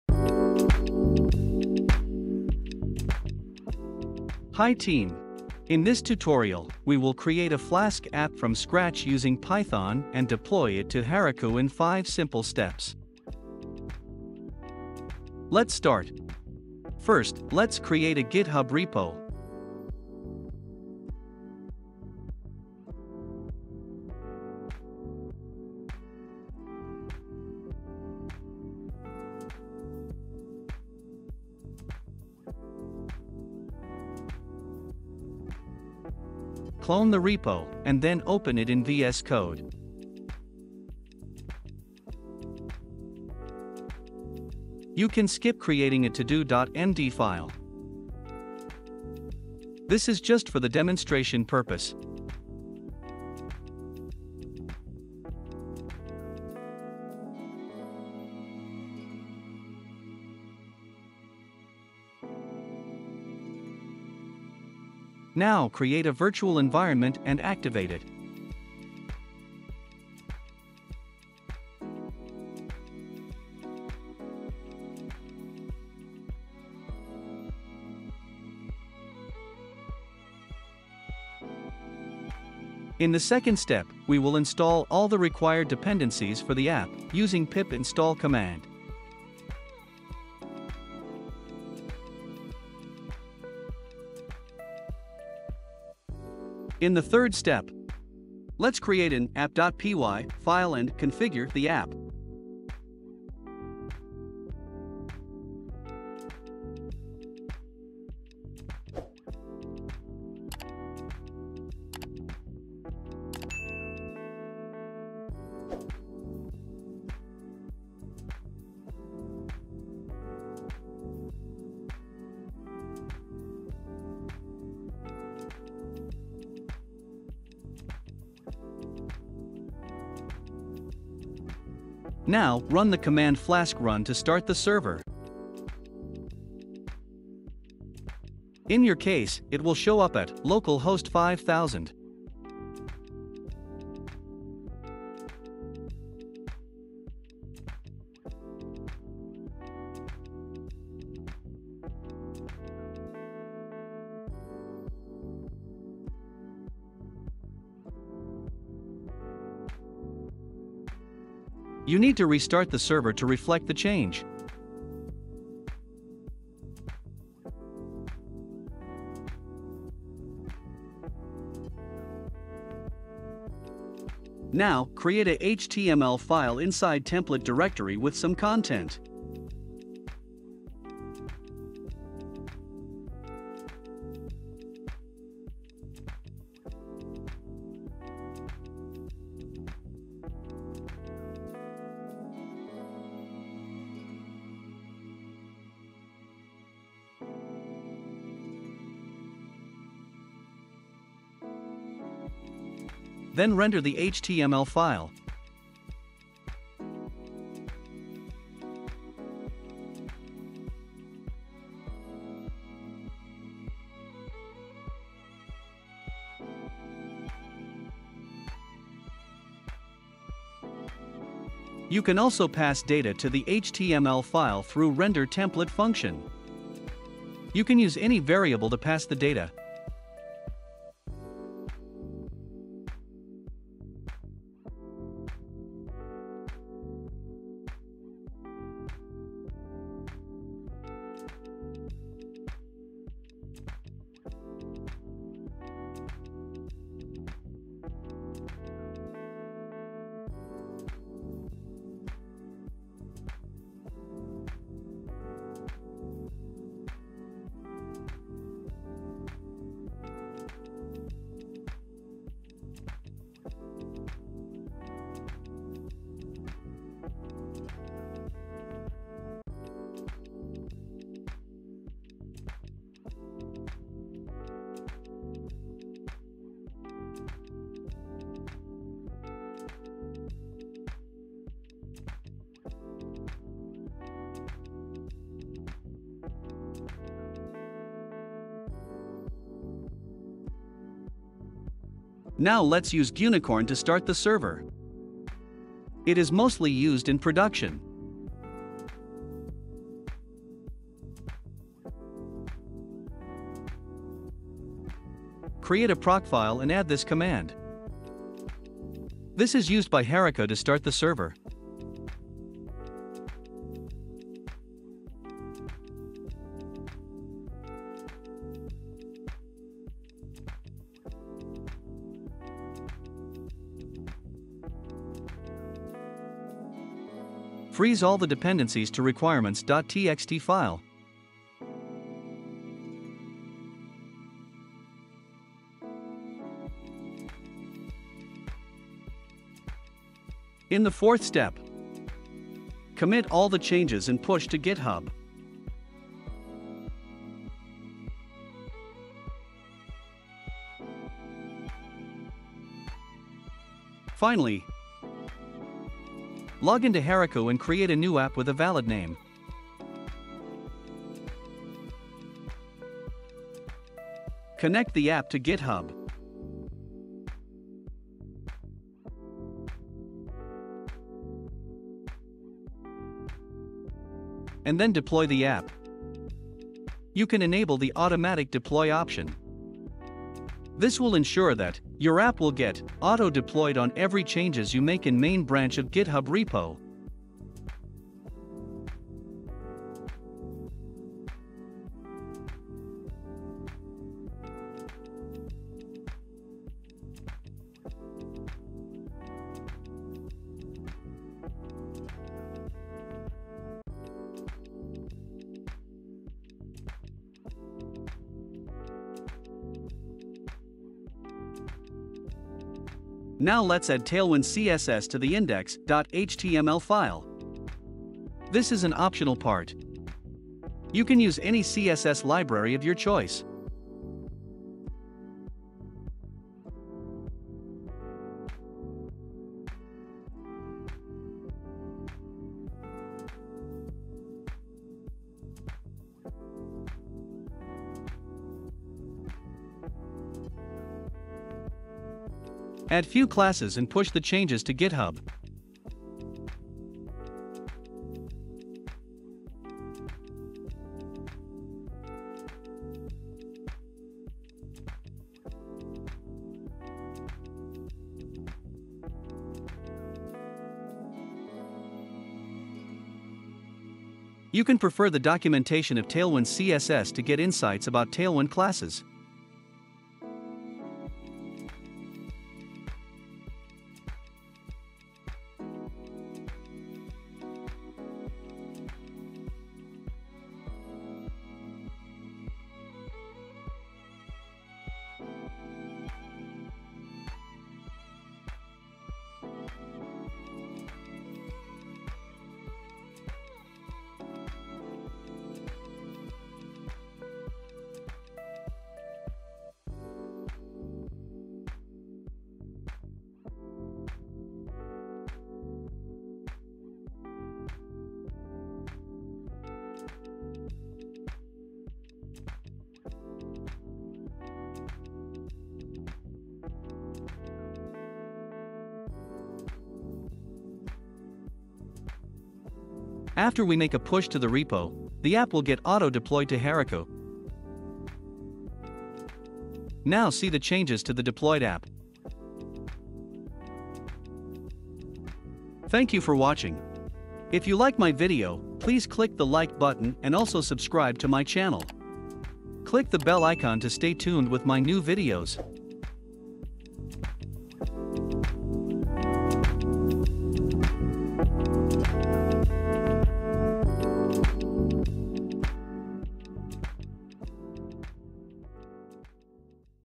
How to create and deploy a Flask app to Heroku in just 5 simple steps ? | BHIMRAJ YADAV
◆Hi 👏 I am Bhimraj Yadav.
In this video, I have created a flask app from scratch using python and deployed it to Heroku in just 5 simple steps.

⏱️ VIDEO CHAPTERS:
00:00  -  Setup Github Repo and Virtual Environment
01:20  -  Install dependencies
01:42  -  Setup Flask App
05:30  -  Push to Github
05:80  -  Deploy to Heroku
06:50  -  Add Tailwind CSS